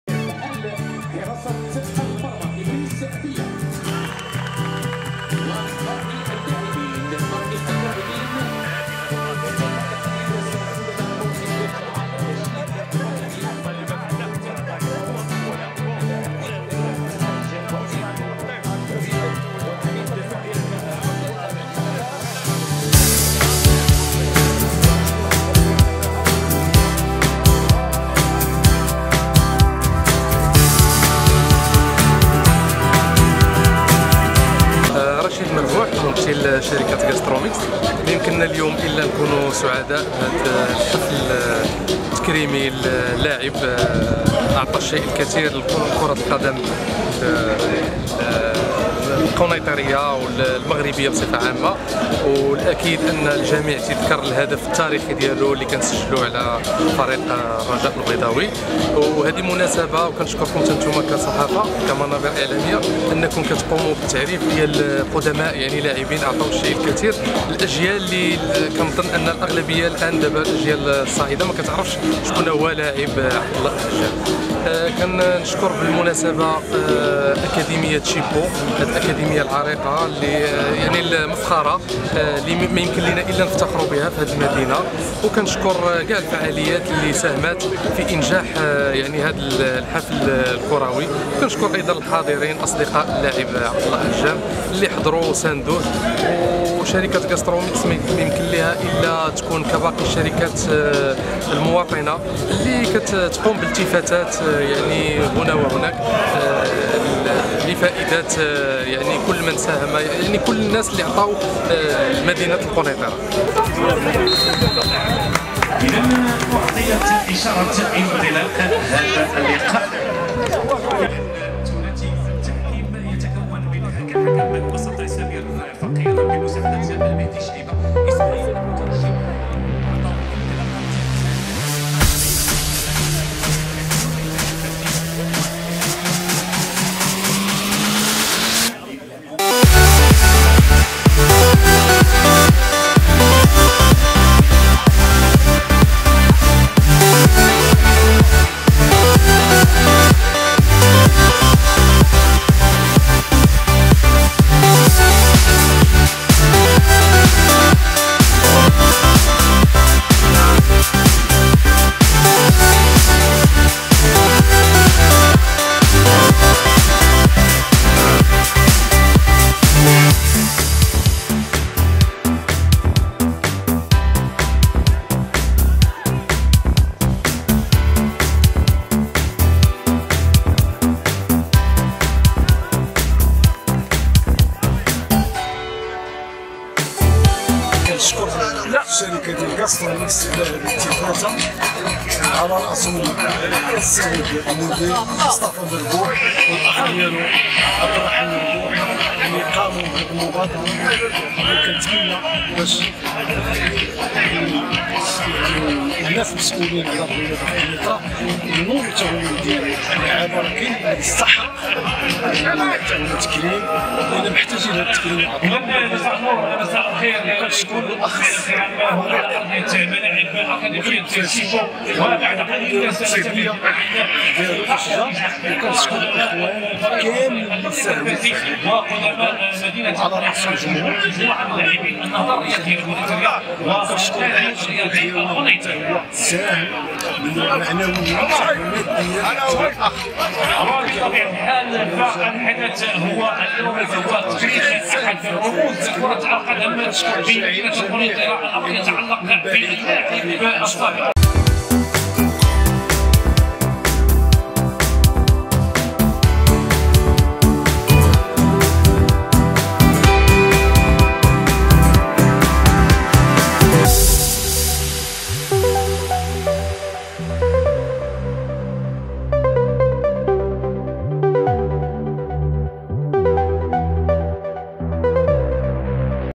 تكريم عبد الله احجام "مرعب حراس المرمى" لاعب النهضة القنيطرية والنادي القنيطري
حفل تكريم لاعب النهضة القنيطرية والنادي القنيطري عبد الله احجام "مرعب حراس المرمى"